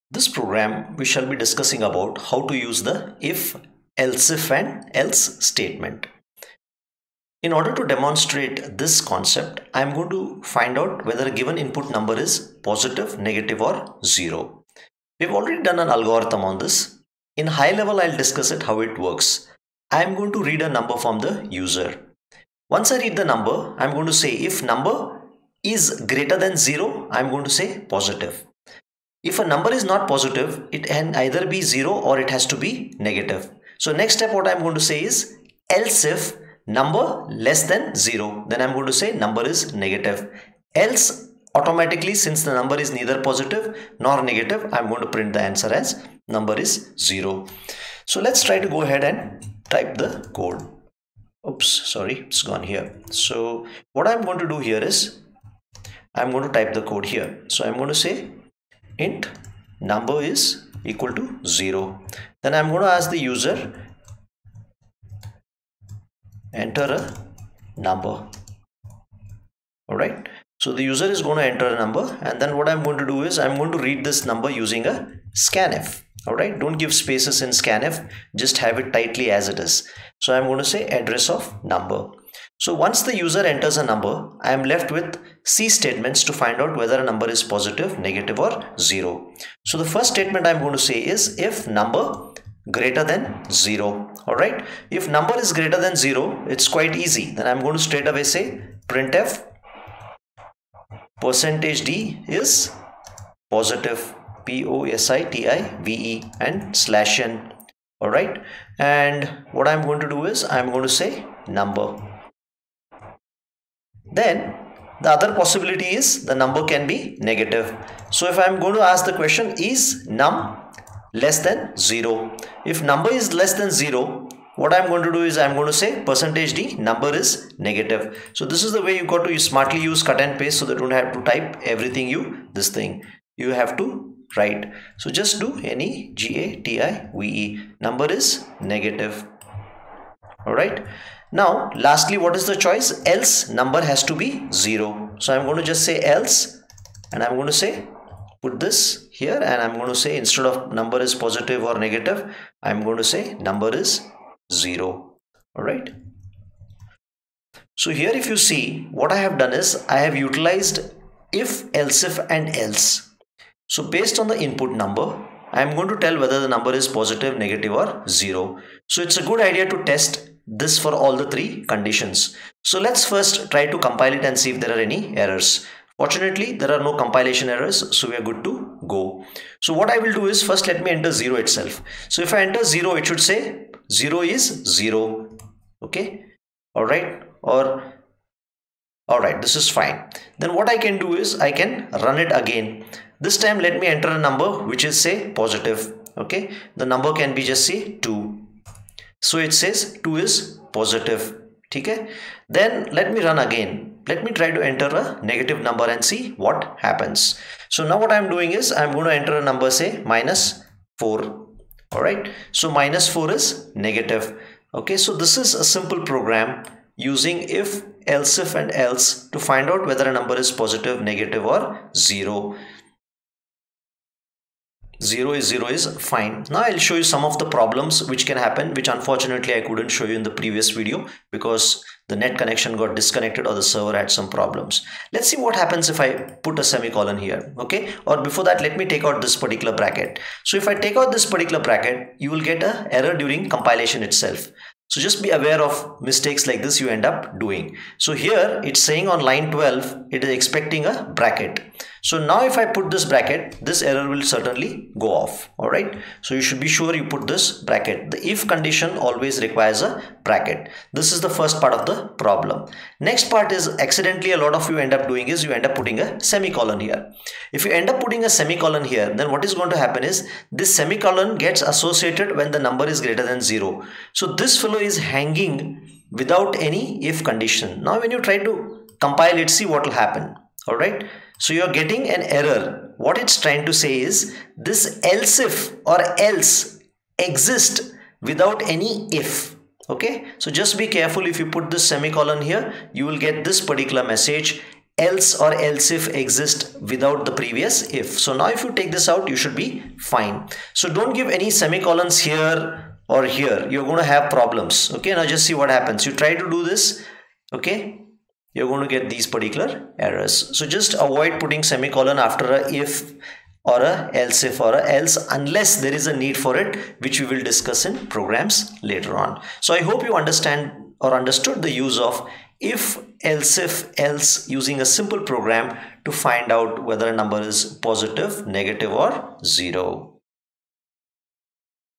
DM 3 if else if else statement Number is Positive Negative or Zero
// Number is Positive Negative or Zero
include stdio.h

int main ()
  int num = 0;

  printf ("Enter a Number \n");
  scanf ("%d", &num);

  if (num GREATER THAN  0)
      printf("%d is a Positive Number \n", num);
  else if (num LESSER THAN  0)
      printf("%d is a Negative Number \n", num);
  else
      printf("%d is a Zero Number \n", num);